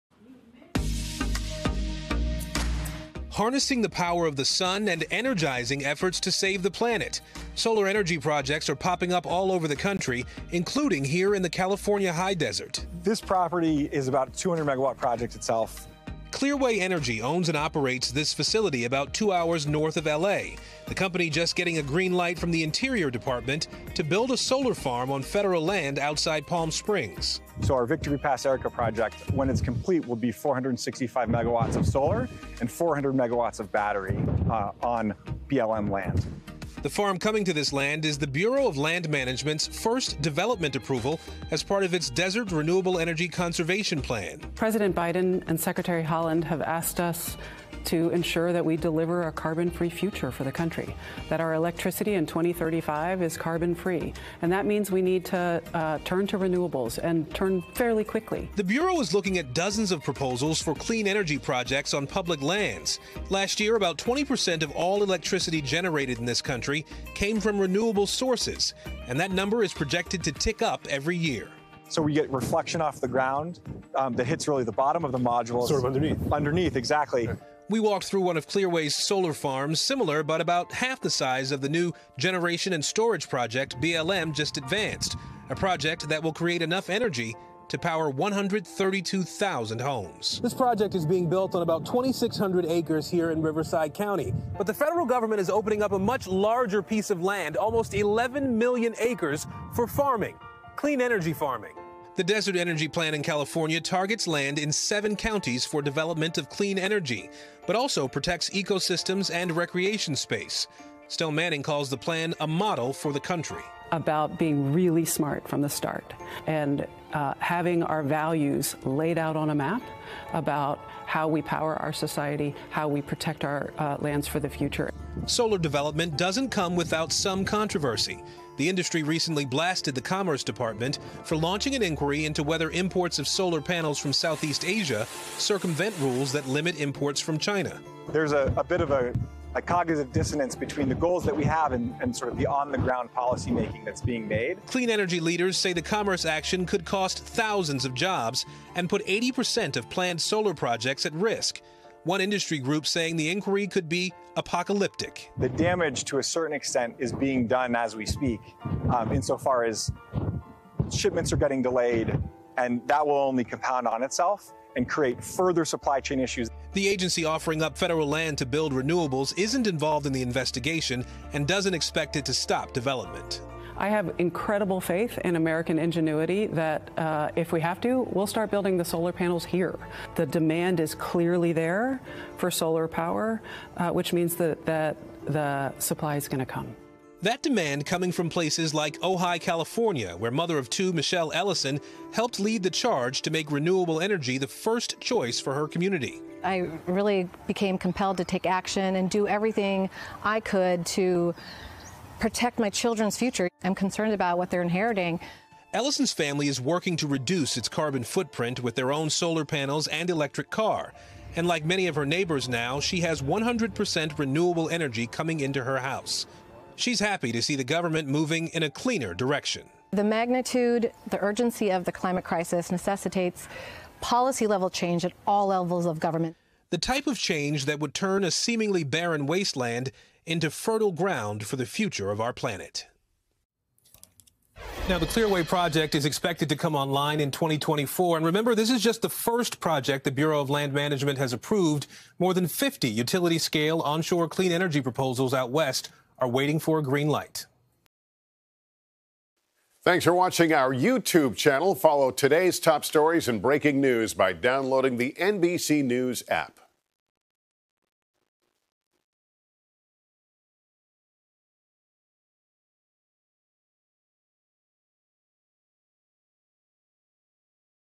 Solar Farms Coming To Federal Land East Of Los Angeles, Can Power 132K Homes
NBC News’ Aaron Gilchrist travels to the California desert, where the first federal land has been approved to become a private solar farm.